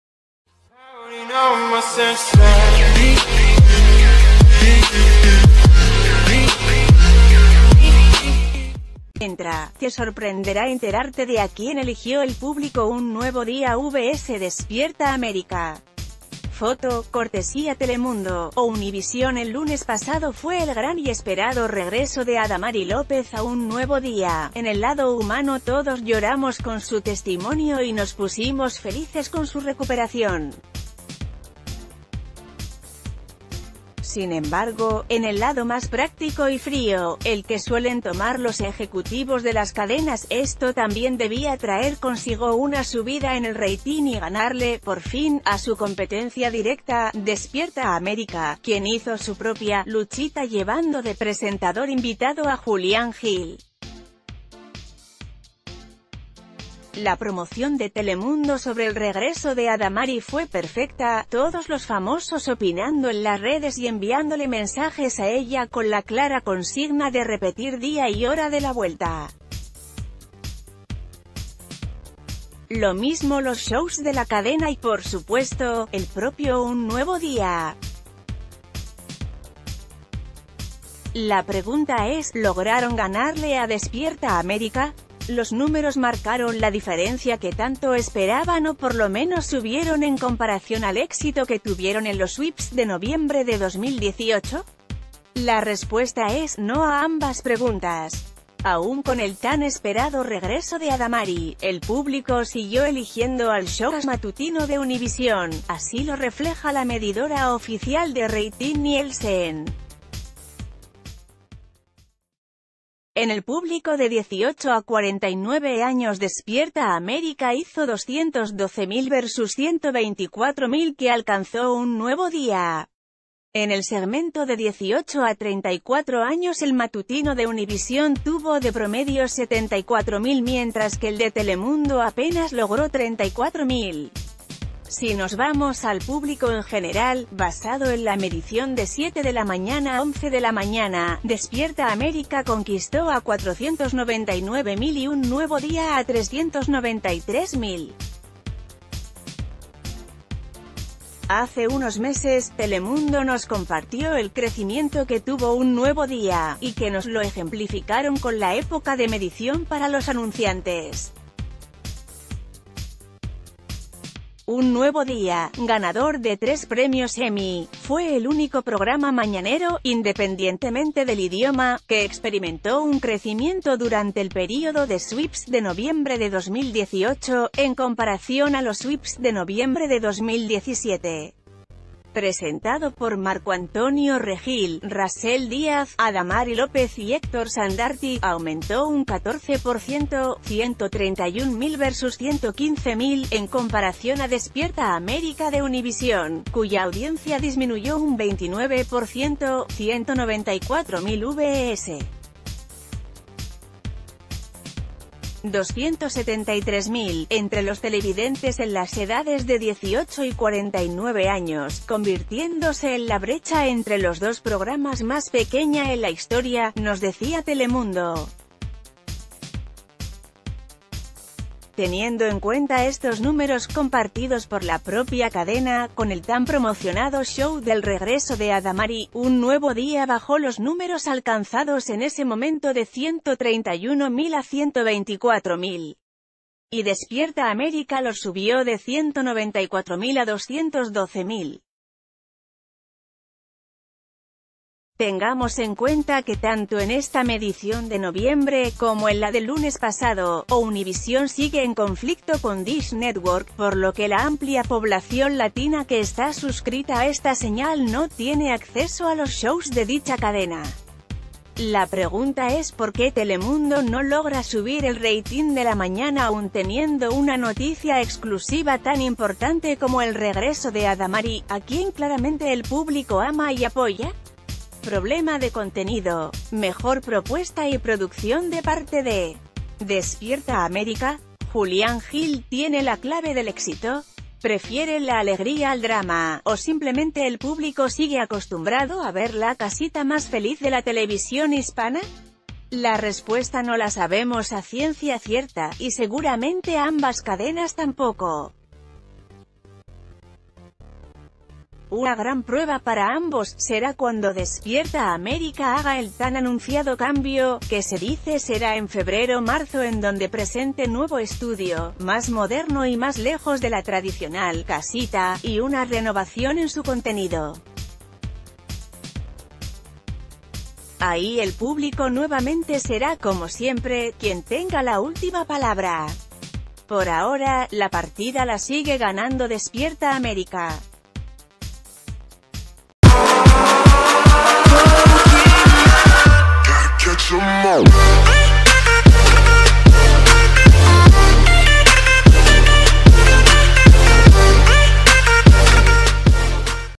Un Nuevo Día vs. Despierta América: El rating con el regreso de Adamari
Entra, te sorprenderá enterarte de a quién eligió el público 'Un Nuevo Día' vs 'Despierta América' Foto: Cortesía Telemundo/Univision El lunes pasado fue el gran y esperado regreso de Adamari López a ‘Un Nuevo Día’… En el lado humano todos lloramos con su testimonio y nos pusimos felices con su recu...